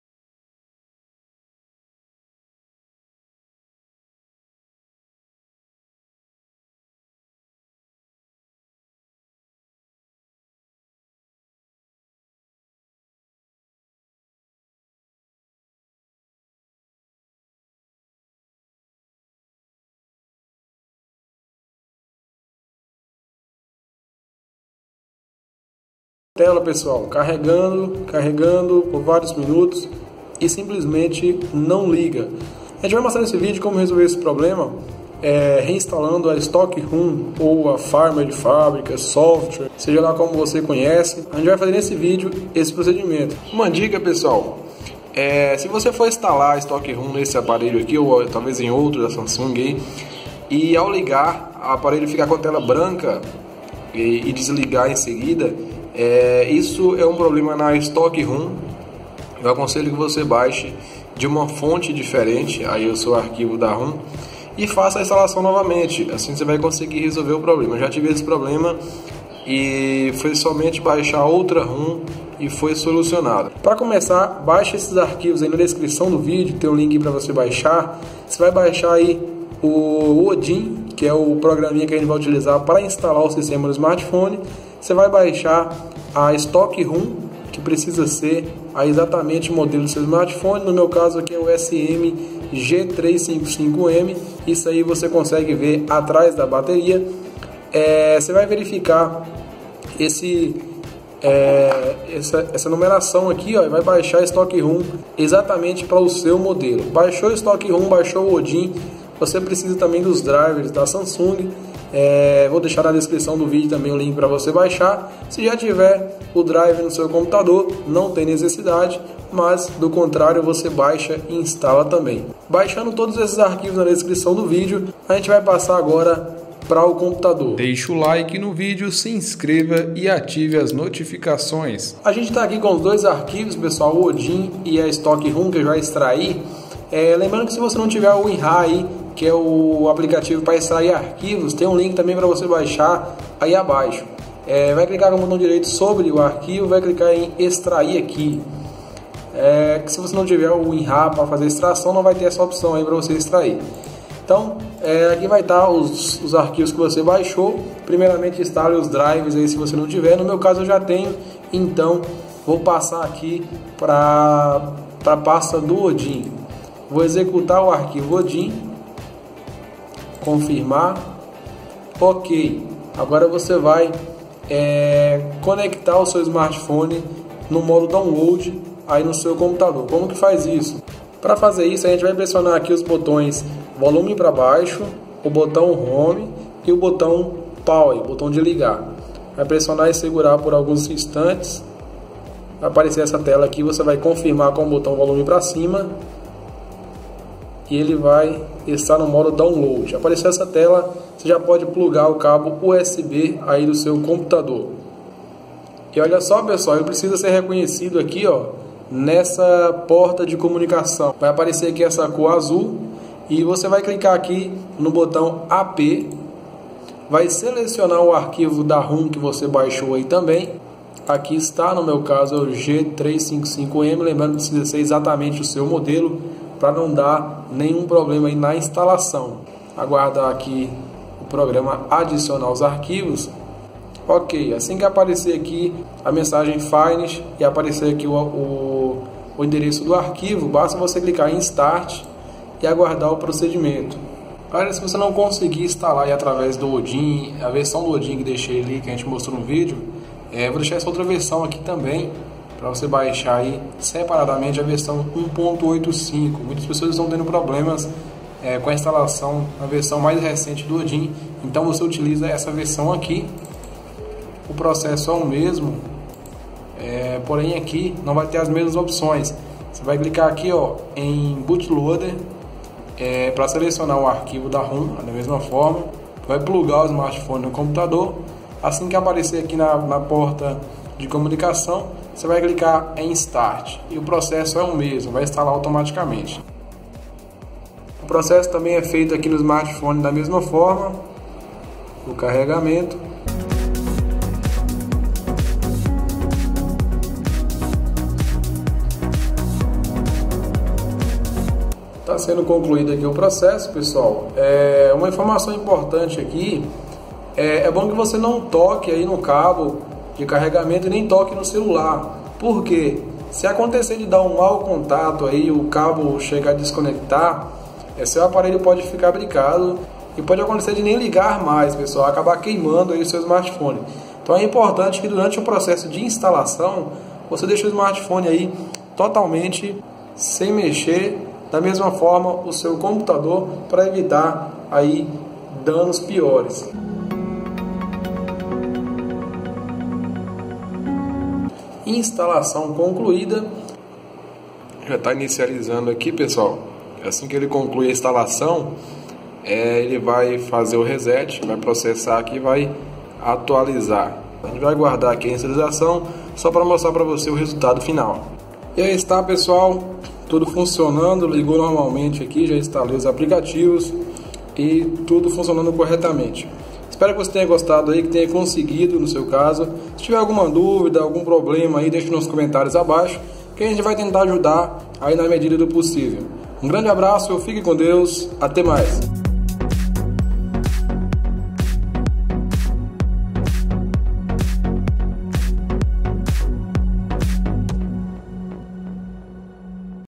SAMSUNG Galaxy NÃO LIGA, fica com a tela BRANCA ou na LOGO (SOFTWARE/STOCKROM) | Guajenet
Celular Samsung Galaxy não liga, fica na tela da logomarca com o nome da Samsung ou a tela vai embranquecendo até ficar toda branca e não liga, veja como resolver.